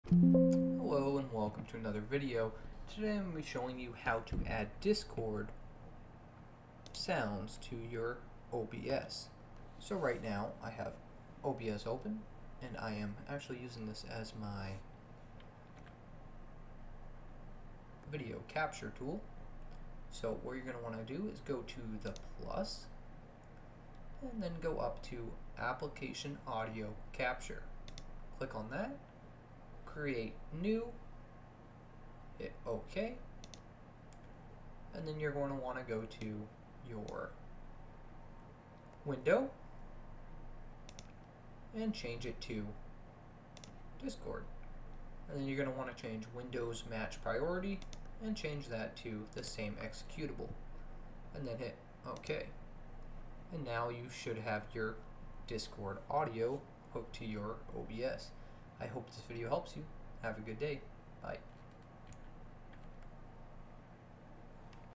How to Add Discord Audio to OBS
I show you how to add Discord Audio to OBS studio so when your recording videos you can allow other peoples voices to be in your recording. Hope it helps.